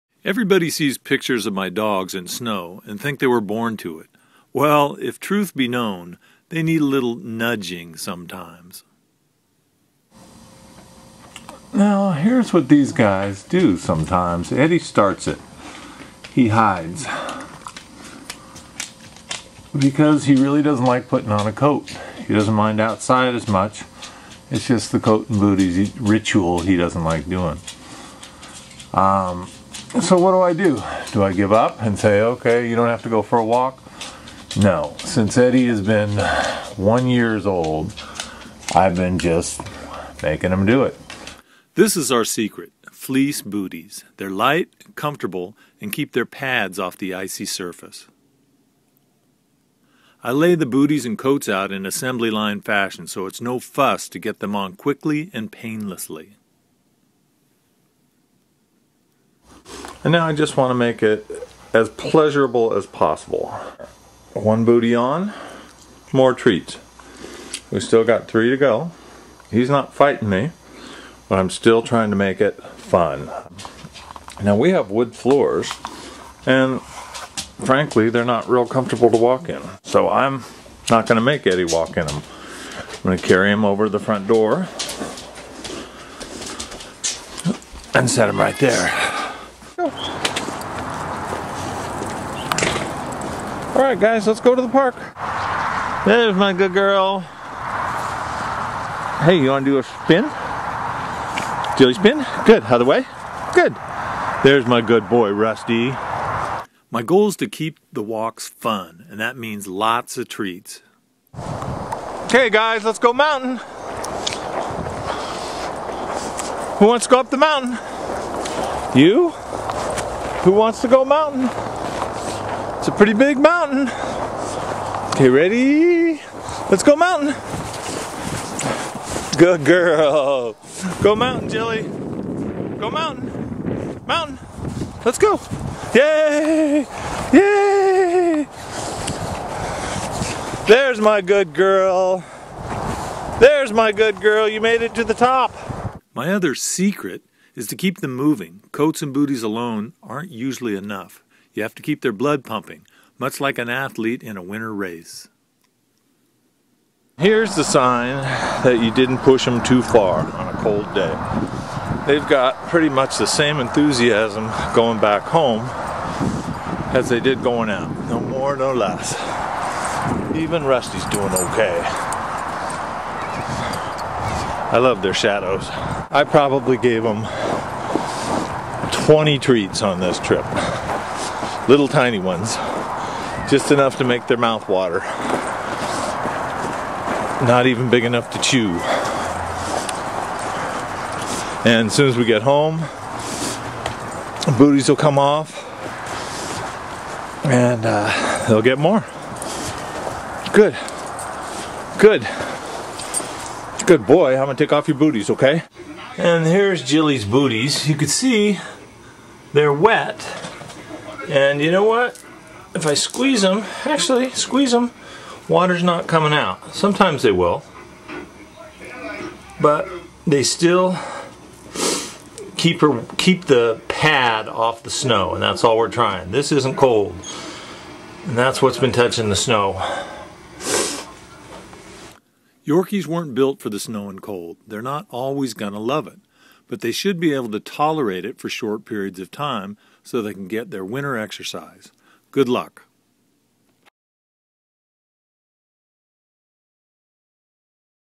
How to get Yorkies to like snow
Some Yorkie owners panic when winter sets in. Yorkies weren't built for the cold, and they often naturally resist it. Here are a few tips on how to turn winter into a pleasant experience for your dogs.